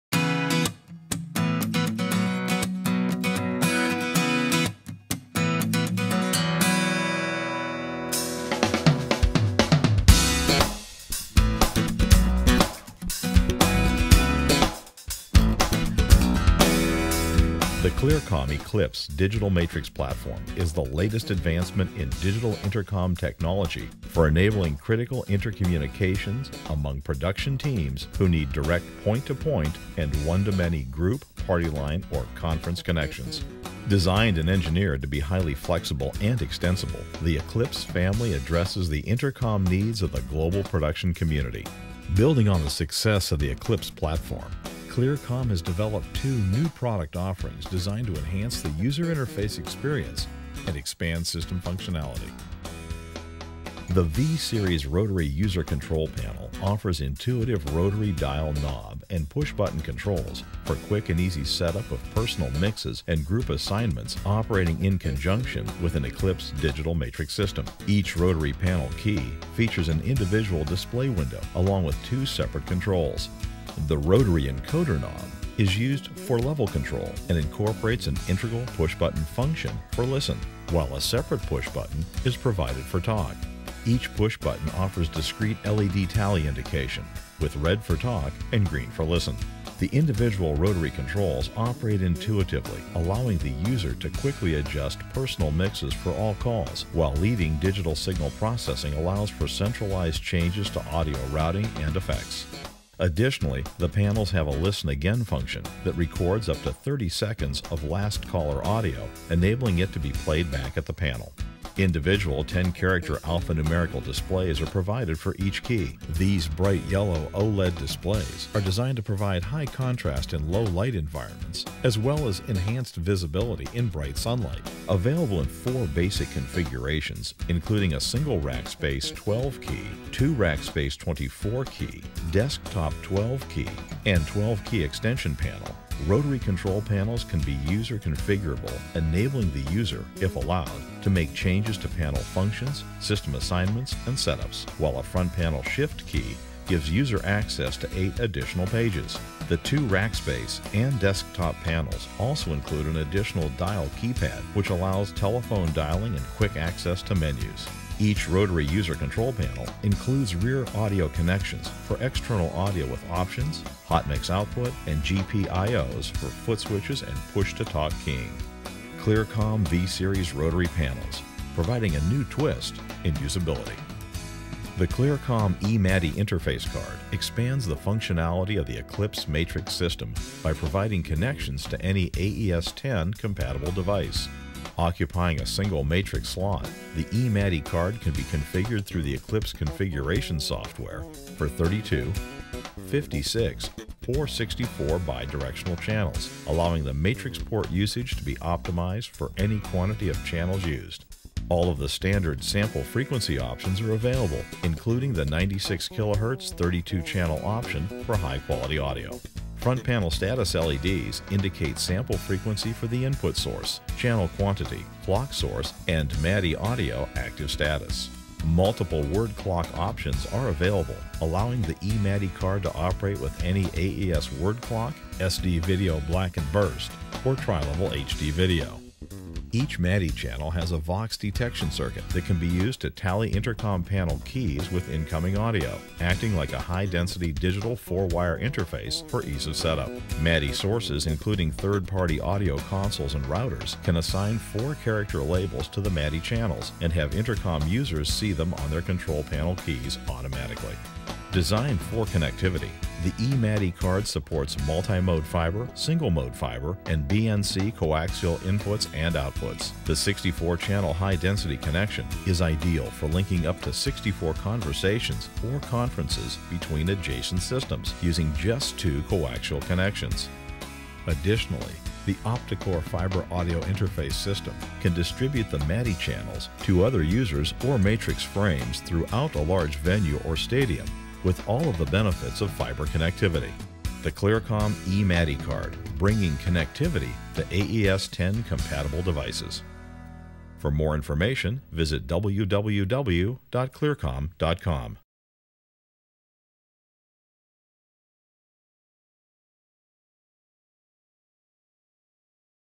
V-Series Rotary Panels & E-MADI64 Card for Eclipse Matrix Intercom
This product introduction video features the new V-Series rotary panels and E-MADI64 card for the Eclipse digital matrix intercom.

V-Series Rotary Panel
Enjoy the advantages of existing V-Series panels in rotary form, with the added benefits of easier and faster audio level adjustments for intercom mixing and IFB assignments. 12- or 24-key rack-mount and 12 key desktop forms available.

E-MADI64 Card
Offering the highest density MADI connection in a single system frame card, each E-MADI64 provides up to 64 bi-directional channels of audio between the Eclipse intercom matrix and any AES10 compatible device. Only available for Eclipse-Omega and Eclipse-Median system frames.